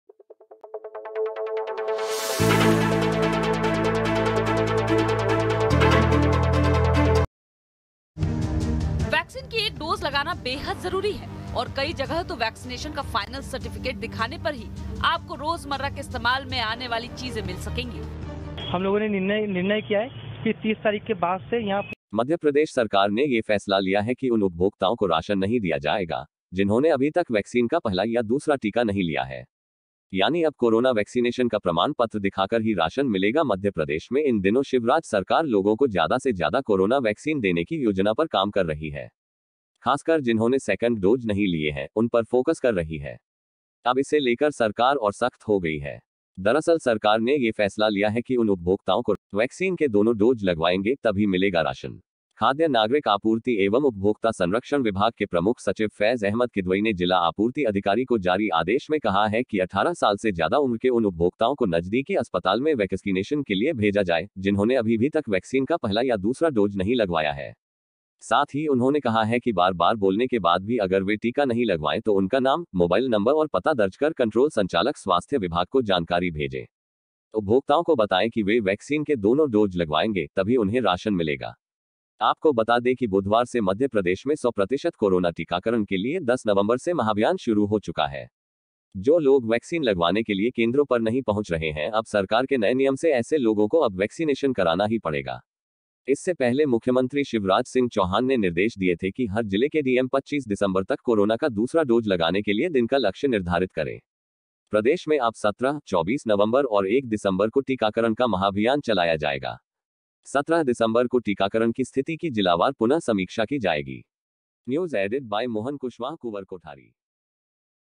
वैक्सीन नहीं तो राशन नहीं मध्यप्रदेश सरकार का फैसला खाद्य एवं नागरिक आपूर्ति विभाग ने दी चेतावनी
सरकार ने ये फैसला लिया है कि उन उपभोक्ताओं को राशन नहीं दिया जाएगा, जिन्होंने अभी तक वैक्सीन का पहला या दूसरा टीका नहीं लिया है. यानी अब कोरोना वैक्सीनेशन का प्रमाण पत्र दिखाकर ही राशन मिलेगा.